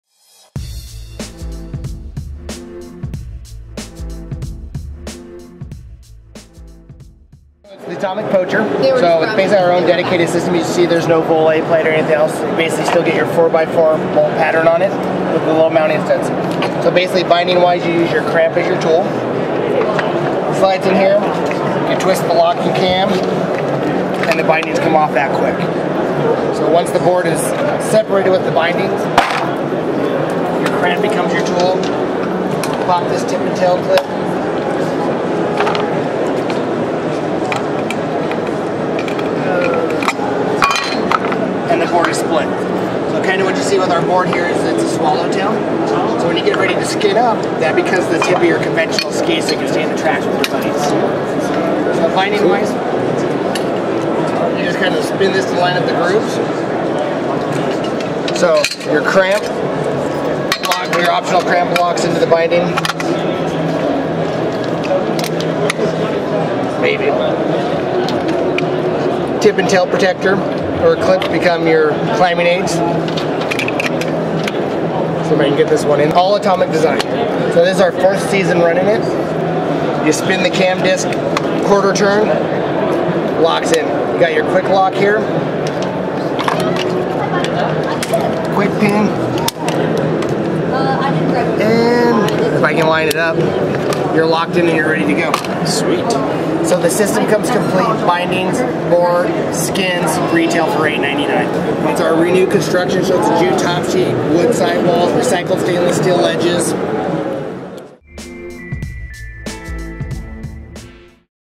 Atomic Snowboards - Poacher Splitboard - Fall 2011 Gear
SIA 2011
2011/2012 Atomic Poacher Swallow tail Splitboard.
Unique split design complete with Atomic's own hardware setup. The crampon becomes the tool to remove the bindings and the board clips, it also serves to lock the bindings in place during ride mode.